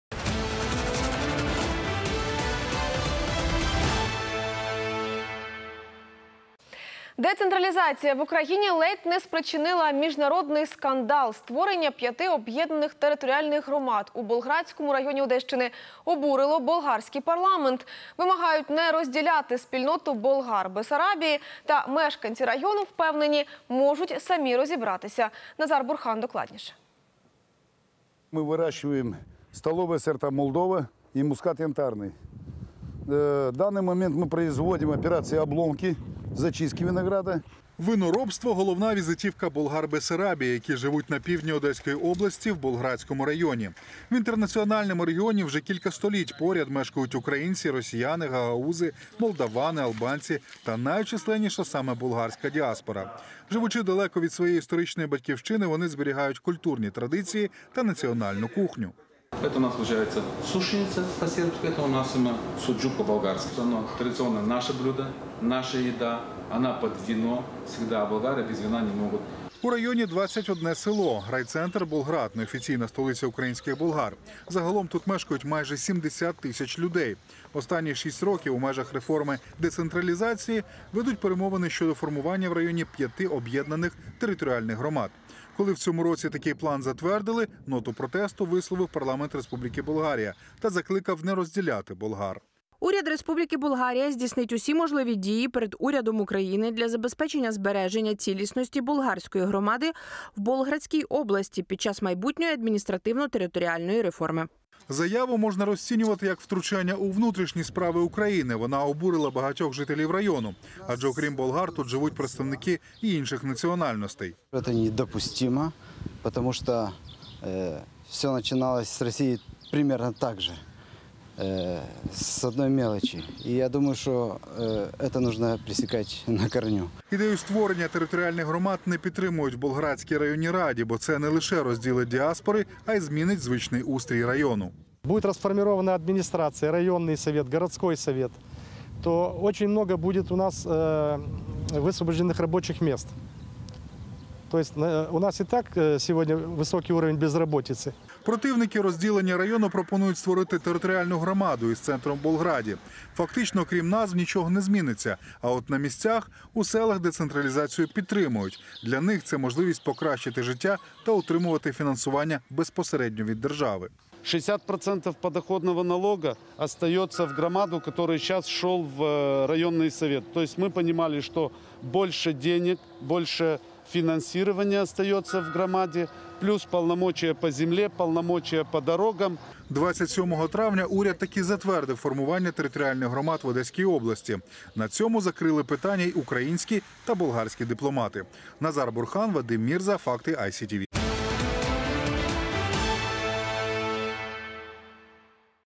Децентрализация в Украине чуть не вызвала международный скандал в Одесской области
Создание пяти объединенных территориальных общин в Болградском районе Одесской области возмутило болгарский парламент. Требуют не разделять сообщество болгар Бессарабии. Но жители района уверены, могут сами разобраться.